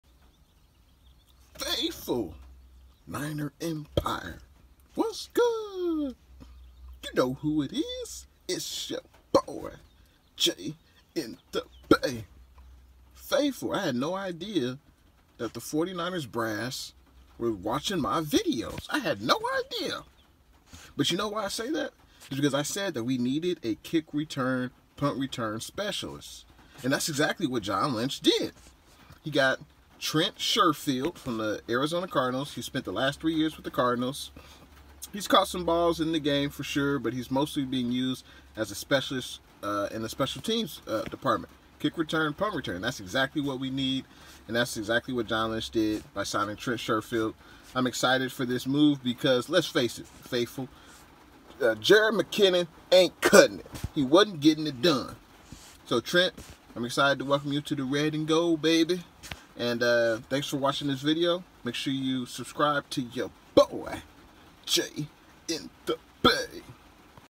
49ers latest roster move: 49ers sign a kick/punt return specialist NFL/49ers Breaking News
49ers/NFL Breaking News: 49ers sign wide receiver Trent Sherfield! He’s expected to serve as a kick and punt return specialist.
J intheBay!